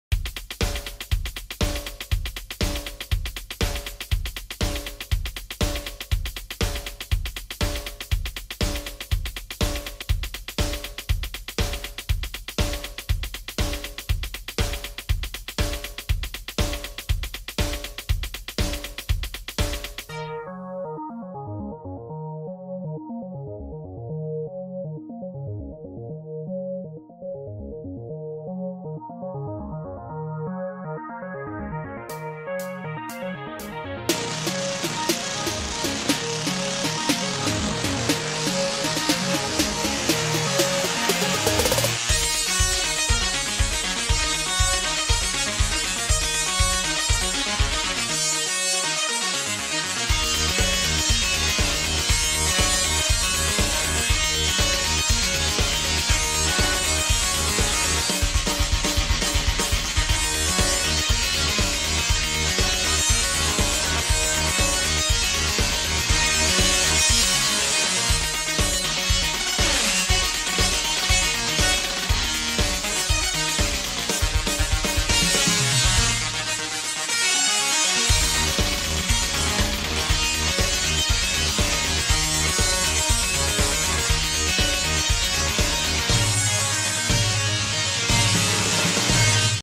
My cute cat is called Jackz walks on a robot vacuum what is this cats Mon Chat sur un aspirateur
My cat is called Jackz walks on a robot vacuum what is this cats Mon Chat  Jackz sur un aspirateur

cat trap game cat trap cat 2022 cat burns huge cat